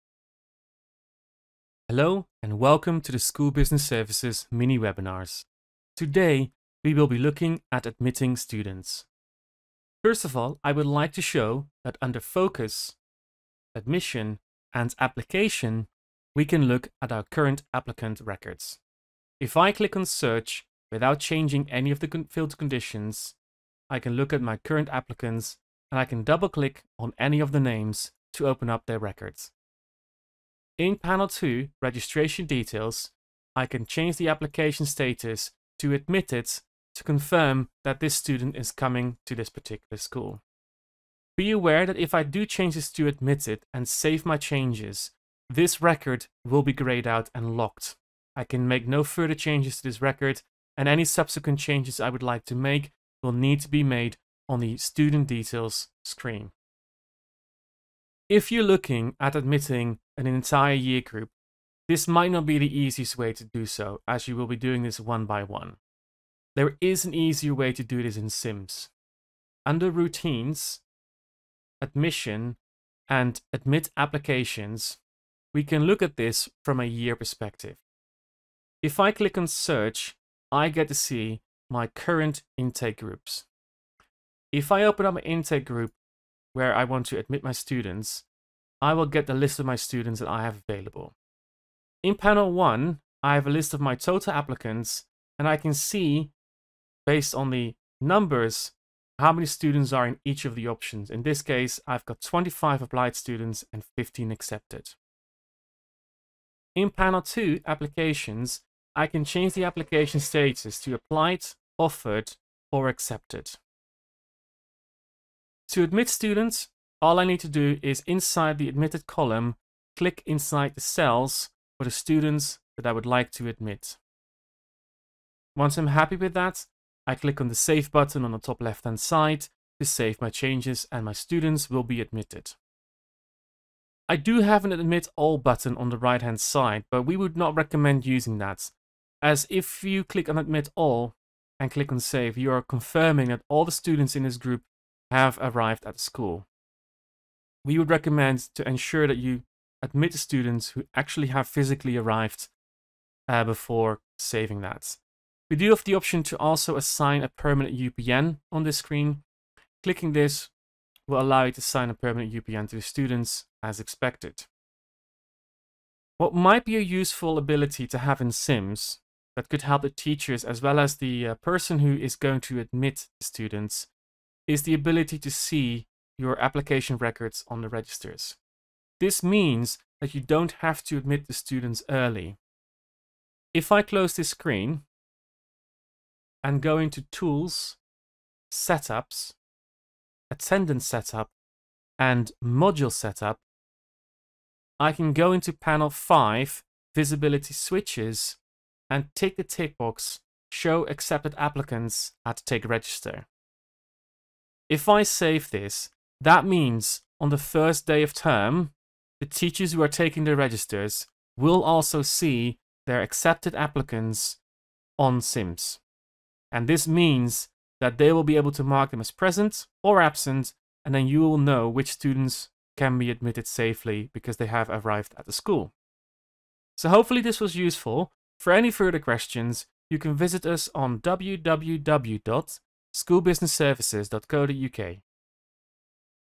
Admitting Students within ESS SIMS
In this video our SIMS Accredited MIS Consultant - Justin Bottinga, shows you how to admit students within ESS SIMS.

You may also be interested in the following videos:

Mini Webinars | End of Year Procedures in SIMS